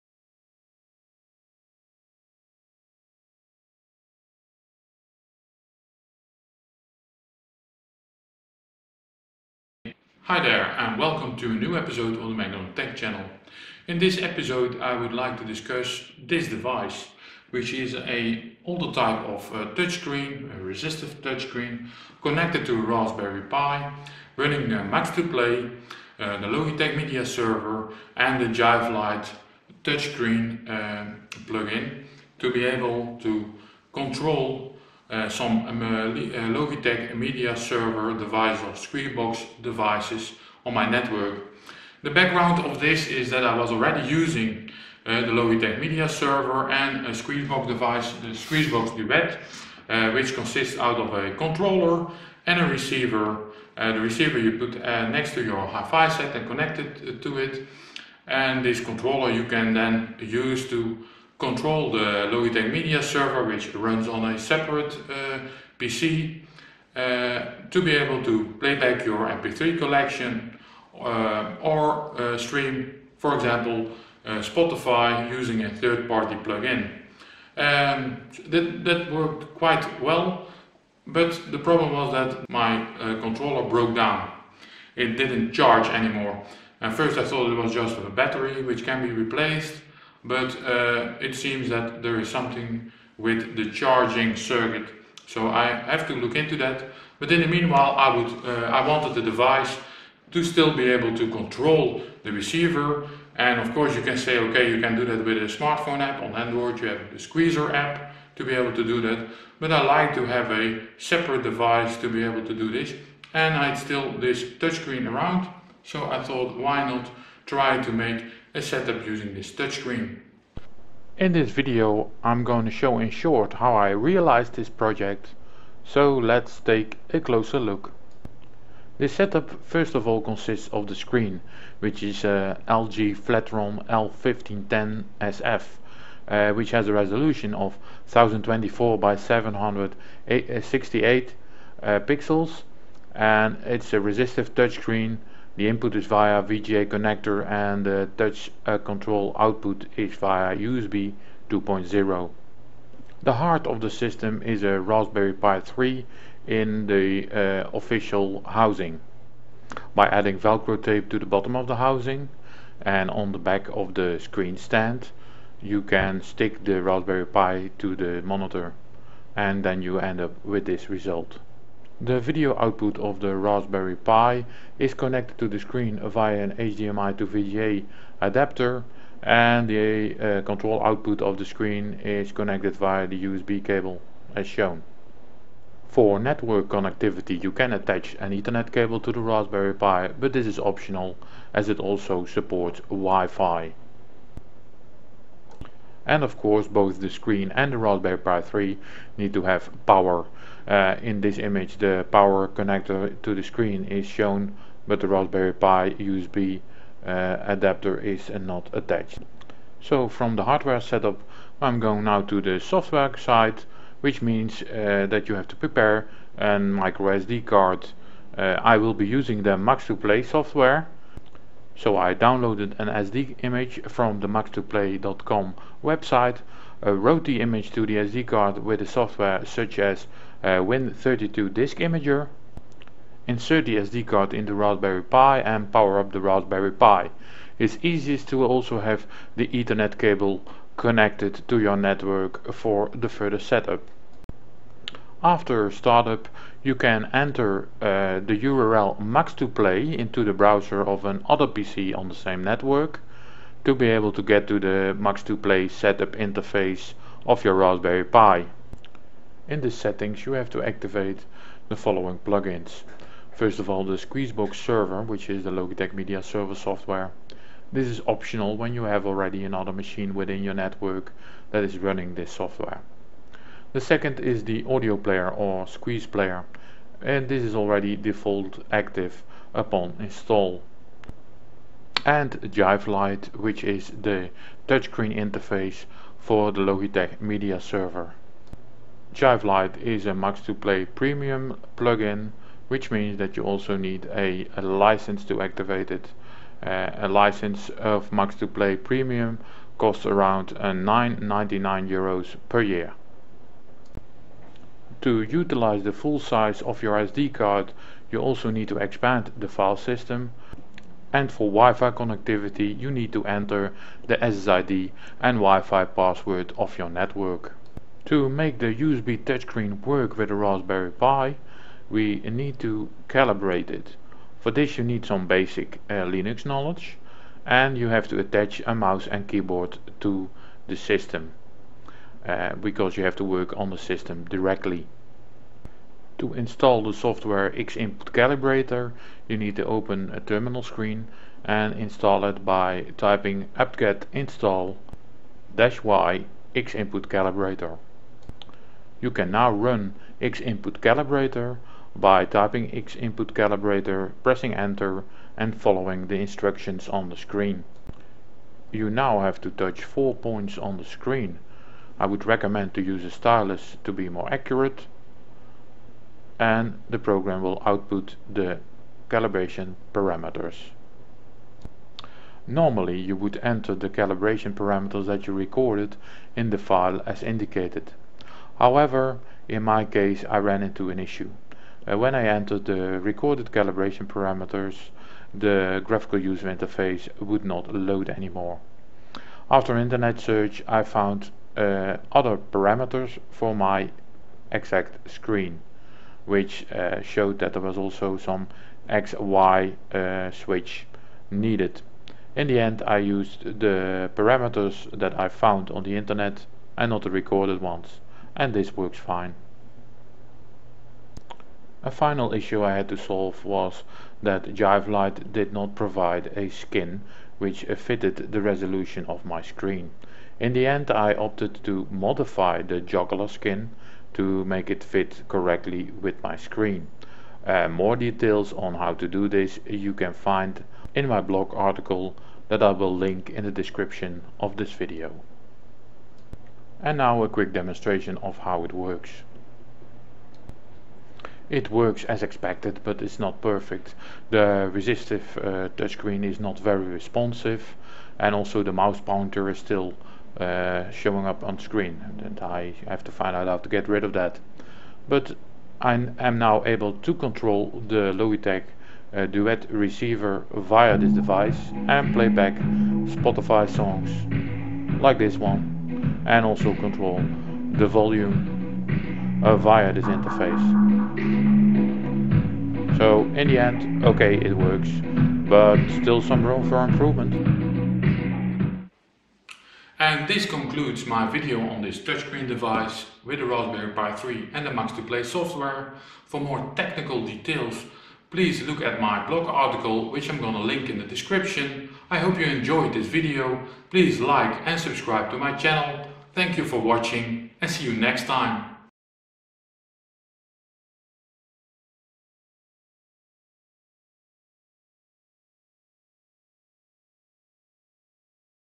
Turning an old LG touchscreen into a music playback device
Using a Raspberry Pi 3, the Max2play audio player OS. Logitech media Server, Squeezeplayer and the JiveLite touchscreen add-on. Using this you can stream audio-files and Spotify music to Squeezebox devices on your network. In my case a Logitech Squeezebox Duet receiver attached to my HiFi set in the living and a Raspberry Pi Zero running Max2play and connected to a speaker in my kitchen.

This video shows in short how to do this. Especially how to make this particular USB touch screen work.

This a full multiroom setup which also allows control via and smartphone app and synchronized playback over multiple devices.